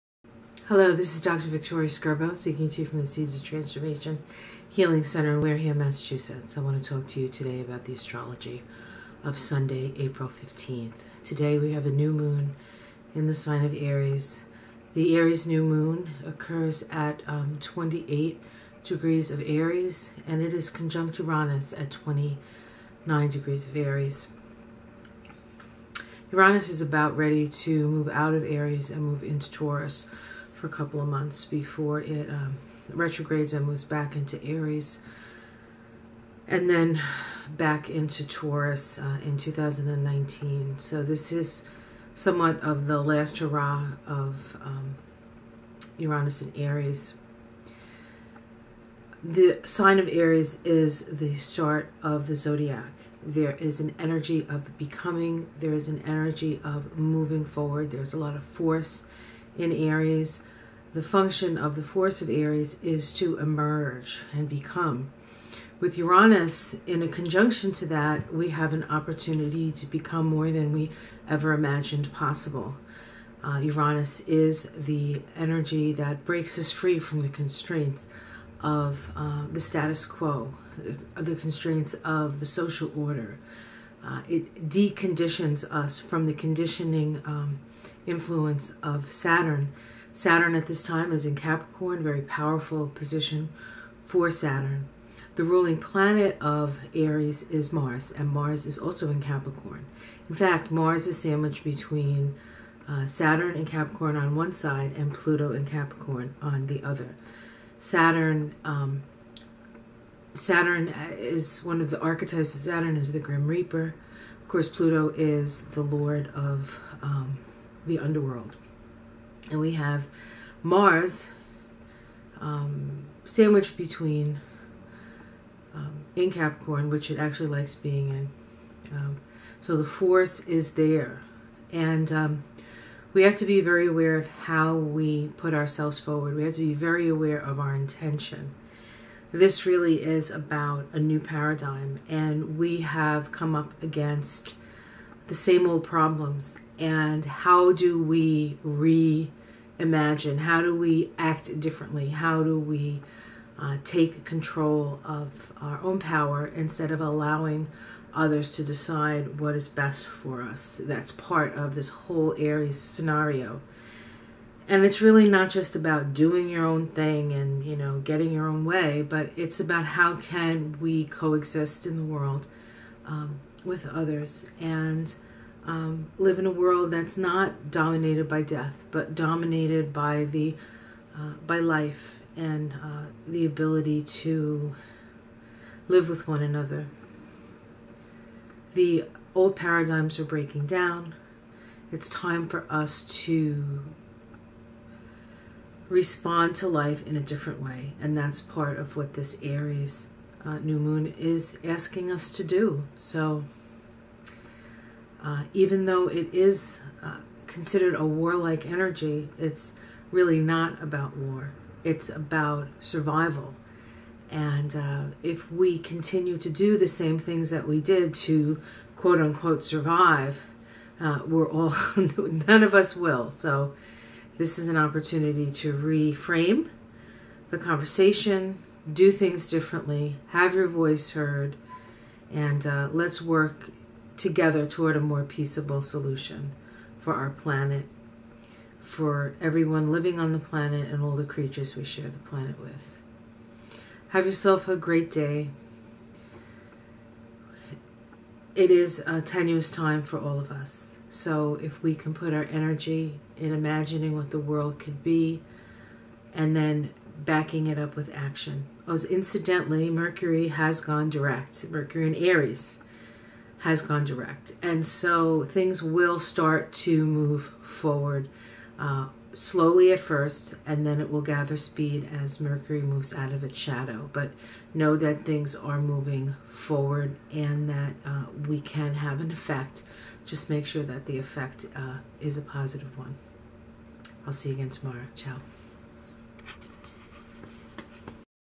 Astrology of April 15 2018
New Moon in Aries conjunct Uranus as Saturn, Mars & Pluto gang up in Capricorn. The Astrology of Sunday, April 15 2018 with Dr. Vicki.